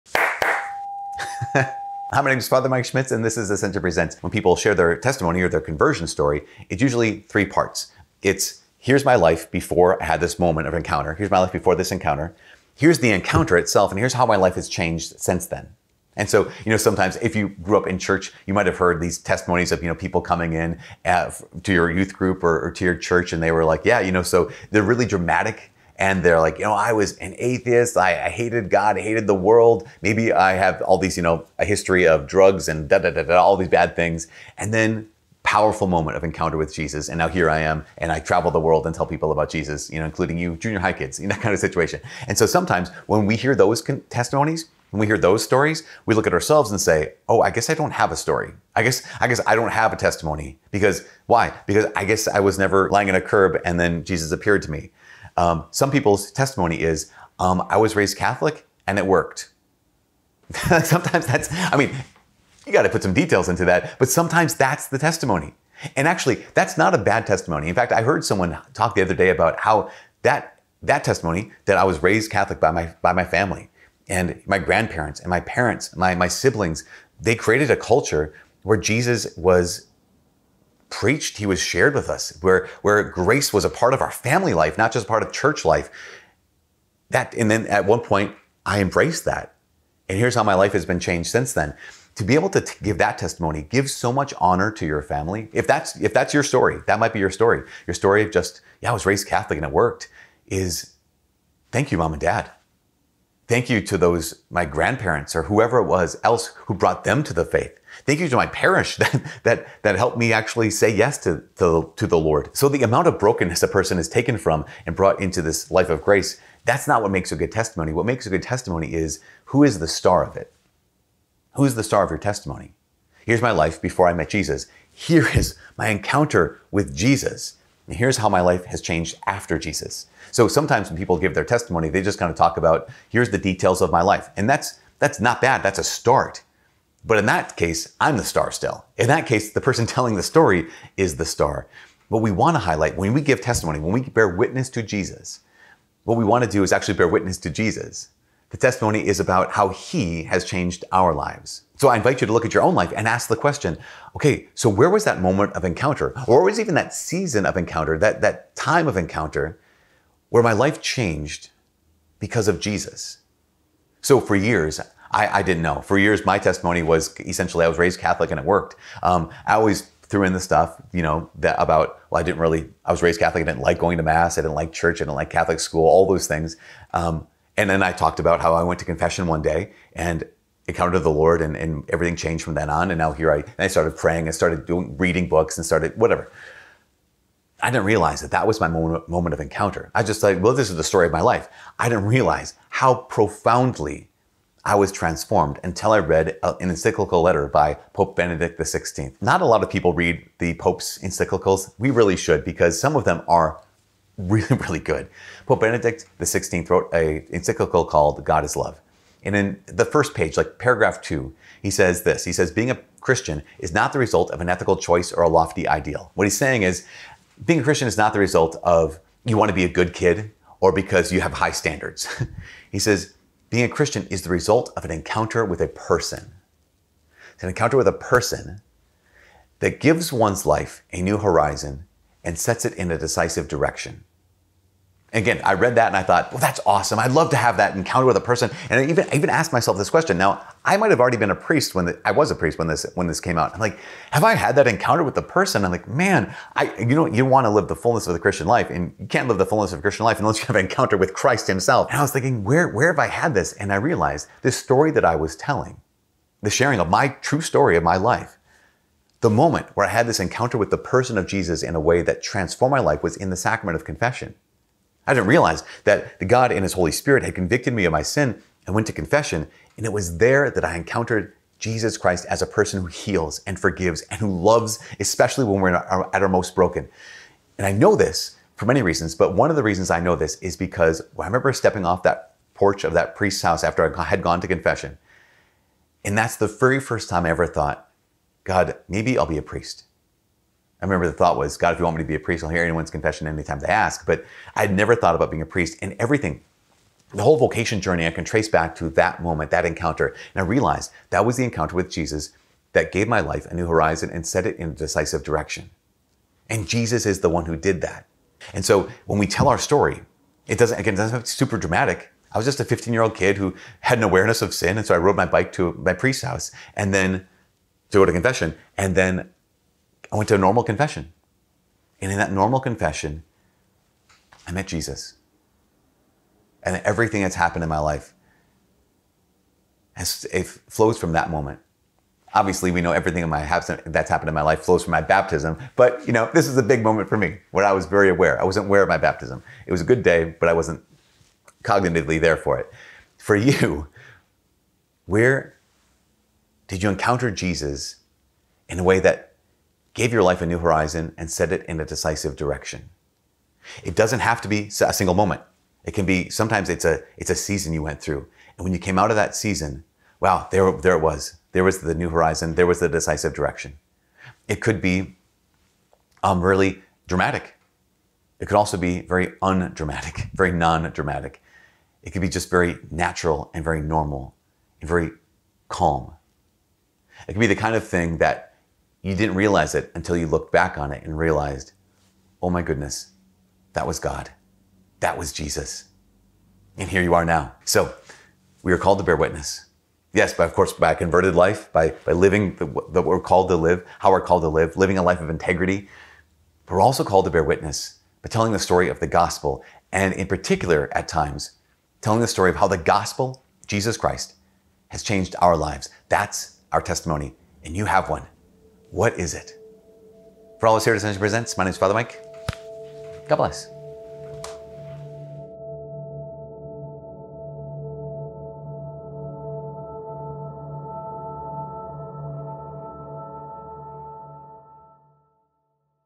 How Do I Know If I've Actually Encountered Jesus?
How do you know you've had an encounter with God? Sometimes it's hard to tell, especially if you were raised in the Faith.

Today, Fr. Mike shares his own conversion story, and encourages us to think of a time or a season where you decided to devote your life to God, because Jesus was there in that time, even if you didn't realize it.

—  SOCIAL MEDIA  —